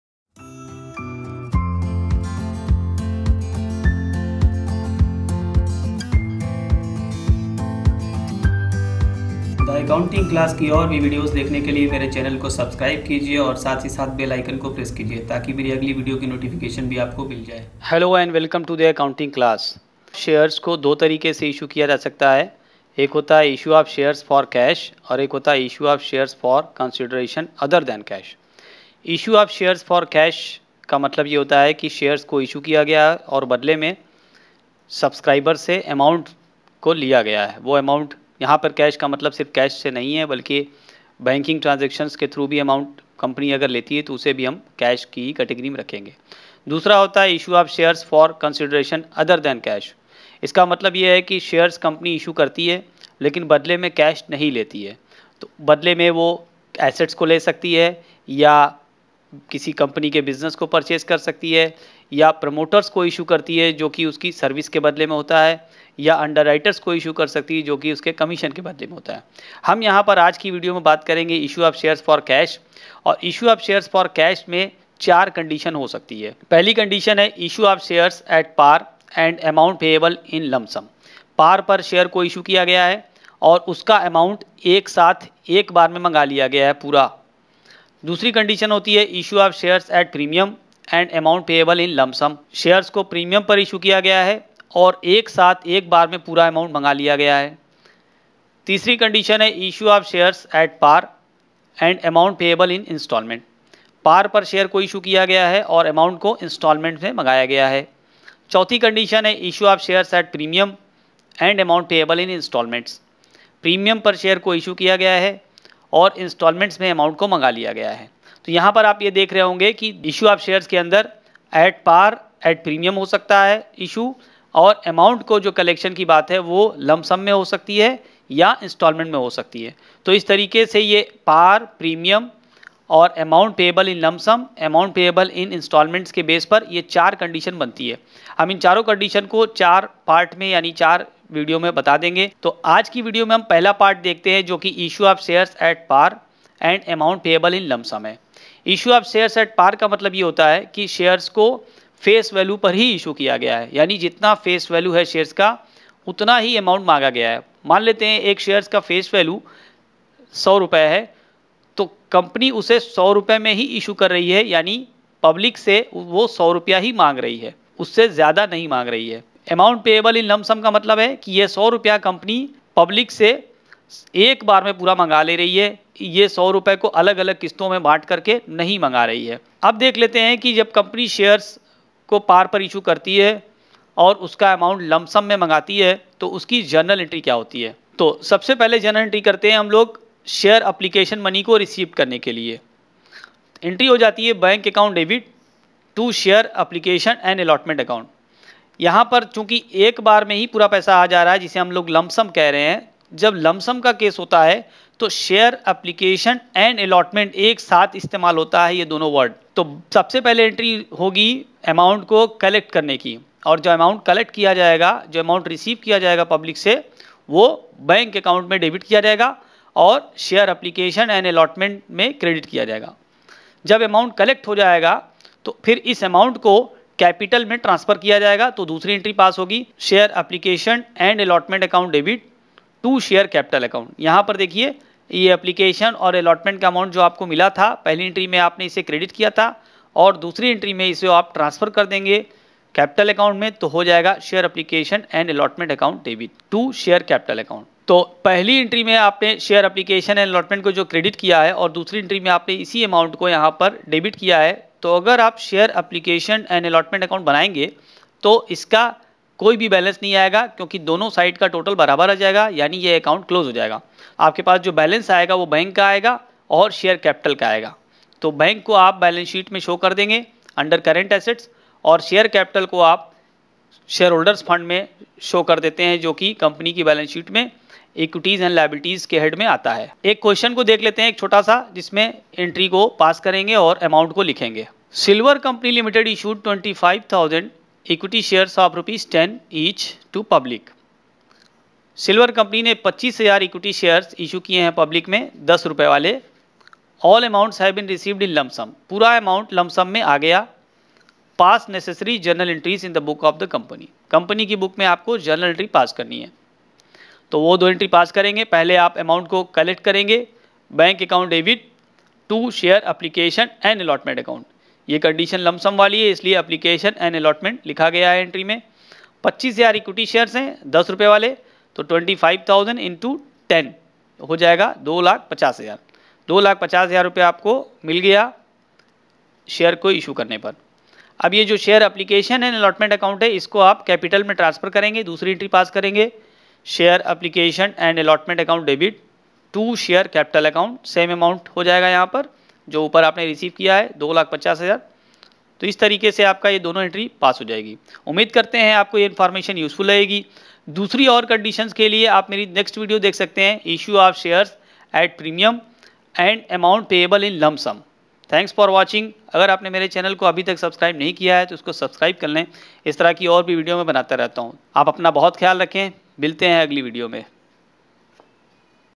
Issue of Shares at Par and Amount Payable in Lump Sum (Hindi)
Watch this video to know journal entries and Practical Question of Issue of Shares at par and amount payable in lump sum.
*Issue of Shares at par and amount payable in lump sum- Concept
*Issue of Shares at par and amount payable in lump sum-Journal Entries
*Issue of shares at par and amount payable in lump sum - Practical Question with solution